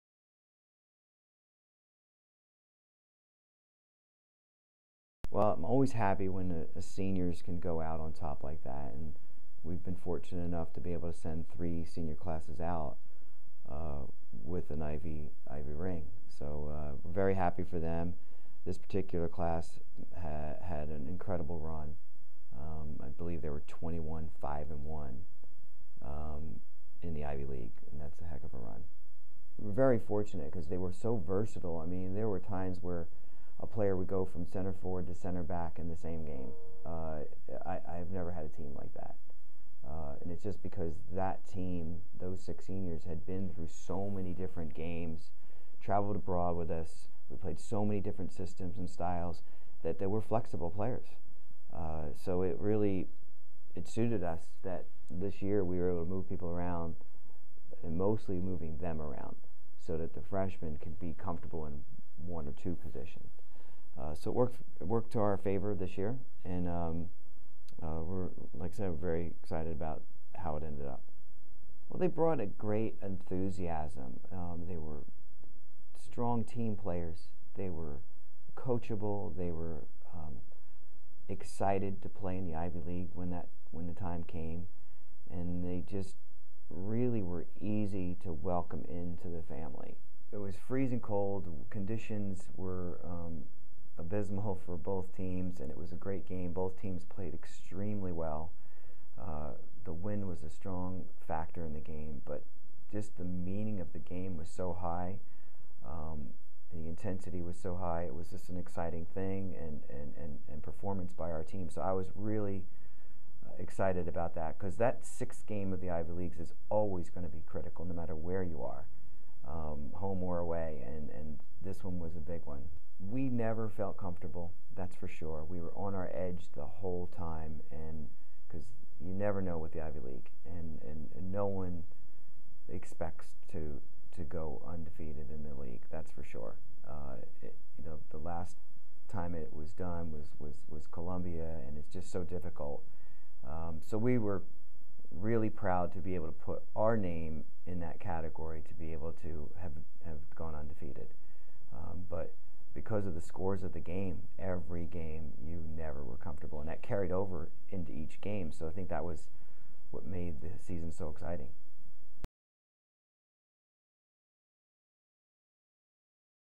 Harvard Women's Soccer 2011 Season Recap
A look back at the 2011 Harvard women's soccer season. The Crimson captured the Ivy League title.